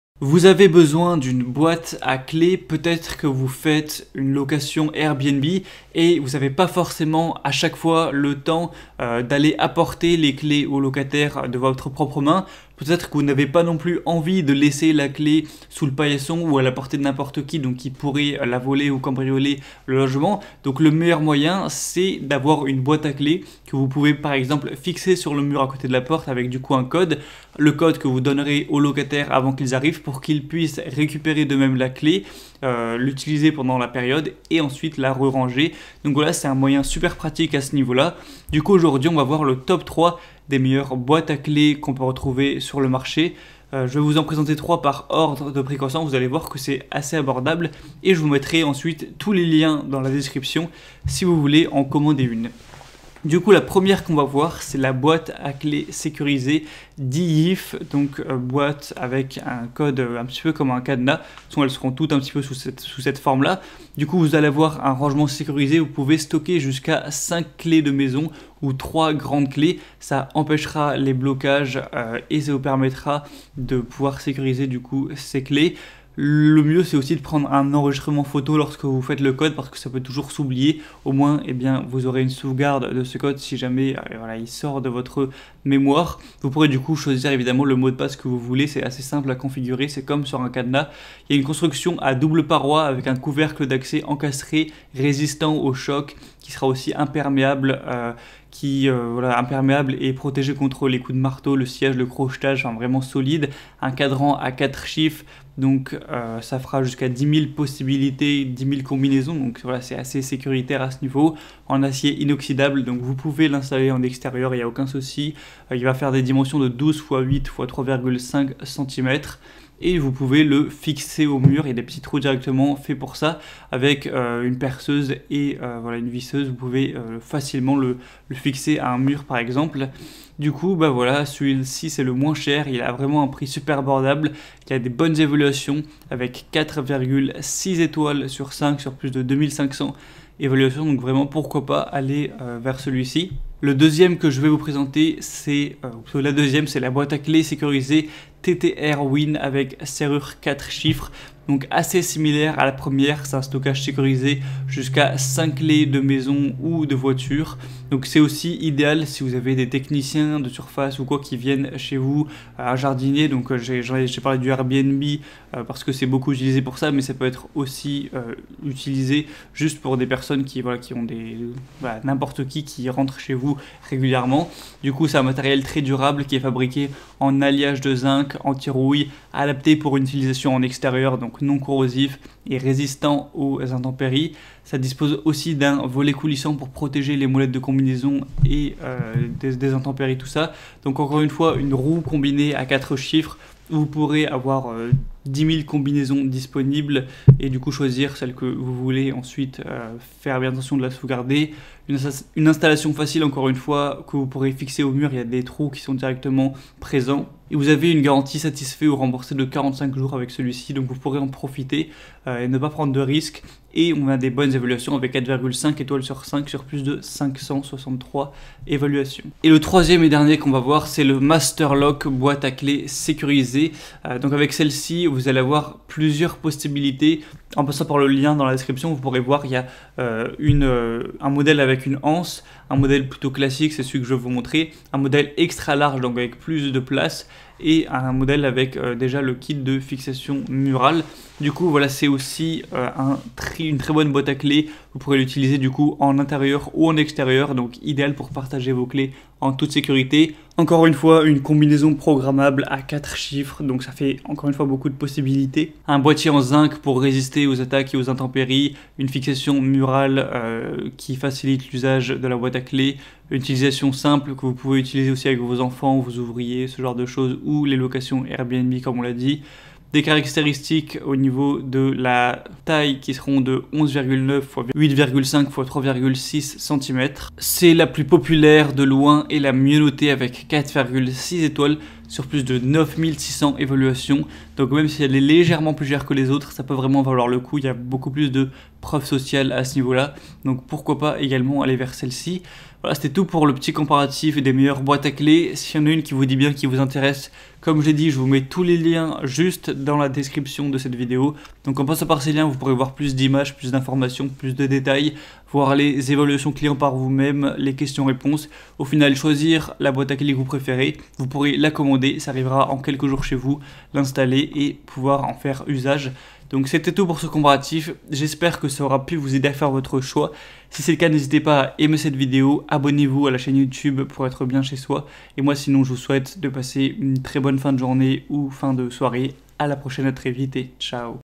TOP 3 : Meilleure Boîte à Clé Sécurisée 2021
⭐Boîte à Clé Sécurisée, Diyife Boîte sûre de serrure d'alliage
⭐Boîte à Clé Sécurisée,TTRWIN avec Serrure 4 Chiffres
⭐MASTER LOCK Boite à clés sécurisée

Pour échanger vos clés facilement et sans prendre de risques offrez vous une boîte à clés sécurisée !

Dans cette vidéo je vous présente le top 3 des meilleures boite à clé sécurisée pas cher et à commander chez soi de l'année 2021 ! Bien Chez Soi participe au programme Amazon Partenaires.